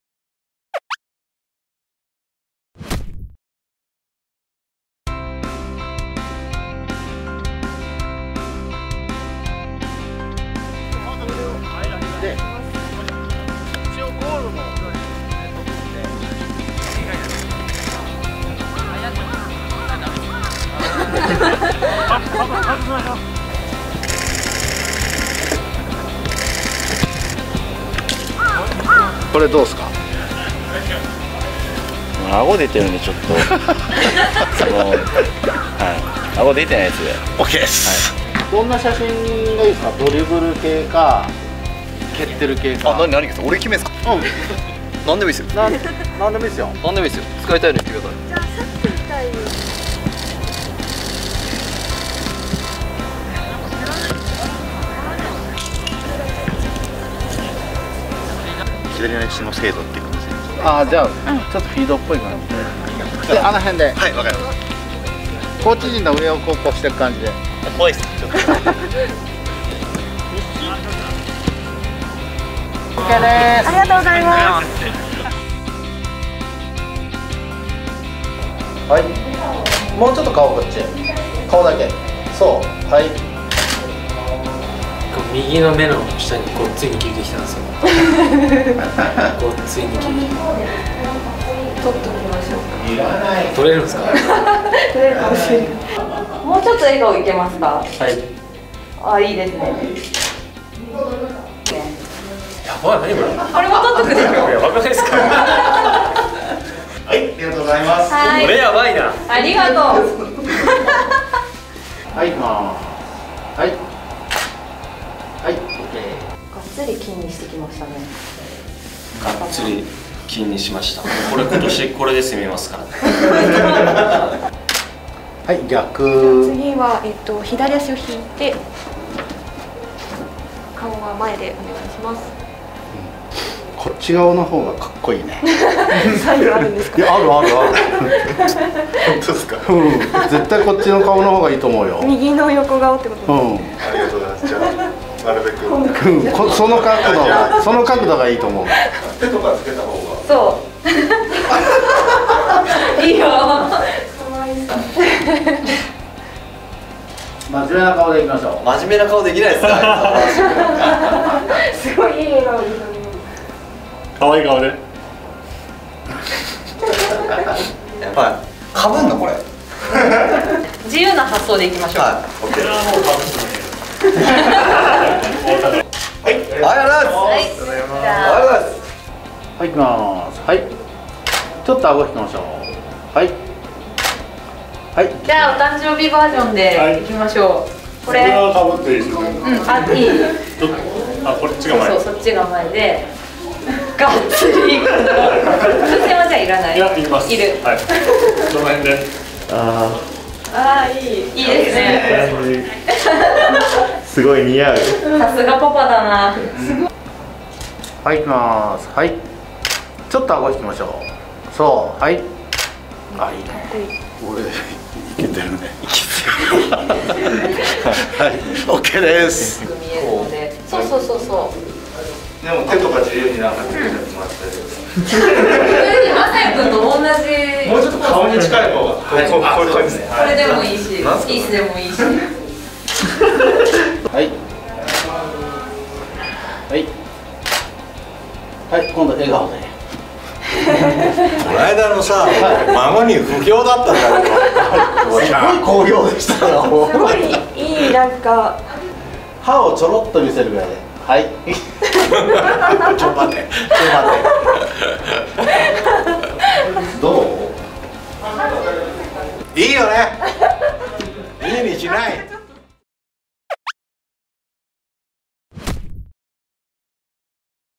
【秘蔵映像】ジュビロ磐田 選手写真撮影 OFF SCENES
1/18(月)に2021シーズンが始動したジュビロ。
今回はプレシーズンの選手写真撮影、裏側の様子をお送りします！
選手たちの素顔が見えるかも・・？
お気に入りのシーンをぜひ、コメントで教えてください！